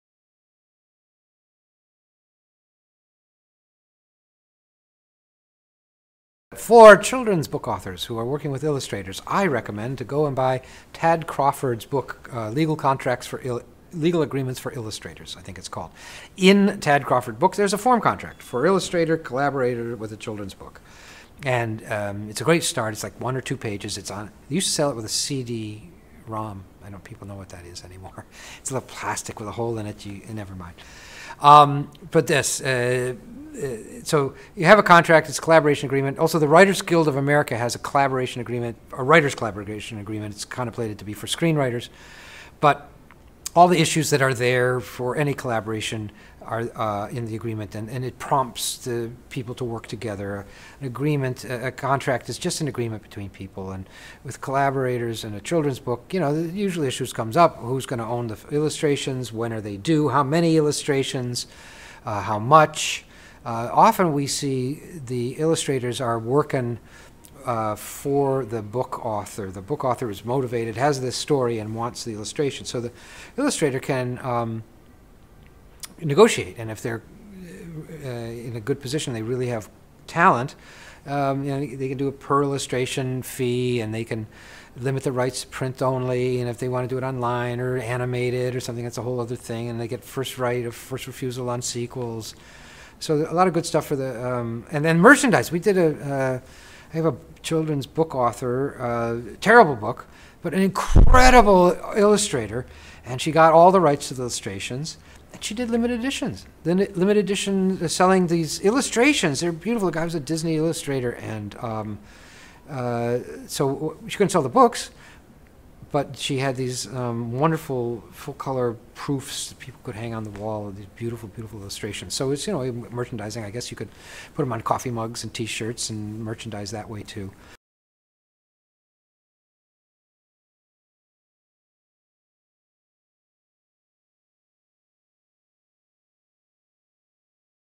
Contract Advice for Authors Entering Into a Collaboration With an Illustrator
Greg Victoroff, Esq., partner at Greg Victoroff & Associates, simply describes contracts as an agreement between people. In the case of authors and illustrators, the agreement may cover who owns the illustrations, when they are due, how many will be due, and so on. Victoroff recommends referring to Tad Crawford’s Business and Legal Forms for Illustrators and the Writers Guild of America Collaboration Agreement to get a good idea of how a contract between an author and illustrator should look. Because the author is usually seeking out the illustrator, the illustrator may have some leverage to negotiate for certain fees and conditions.